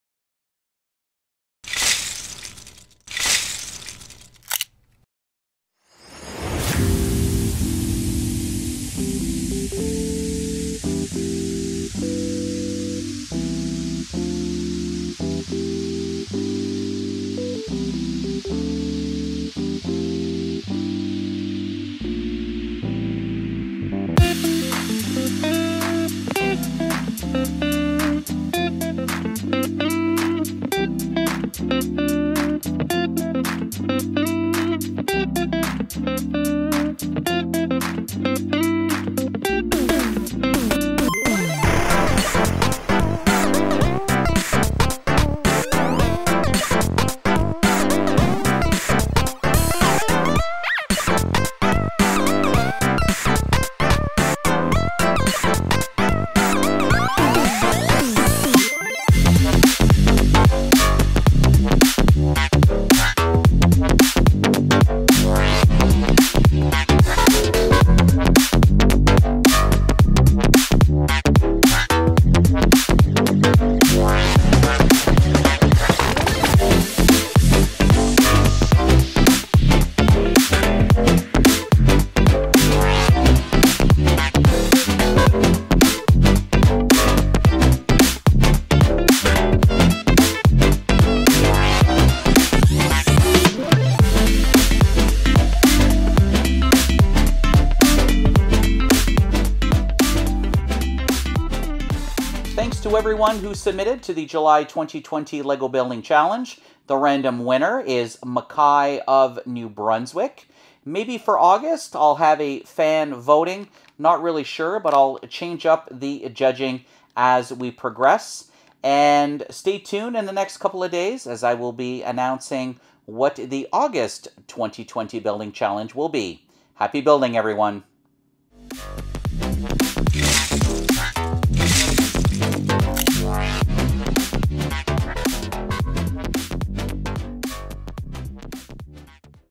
JULY 2020 Lego Building Challenge Submissions
In this video I showcase the submissions I received for the July 2020 Lego Building Challenge. Each month I announce a new challenge and participants have until the 21st to submit them. Then at the end of the month I do a video montage of all the submissions and announce a random winner !!! That person gets a free CMF in the mail from BRIXTOYZ
The new challenge is announced on my channel the first of each month !!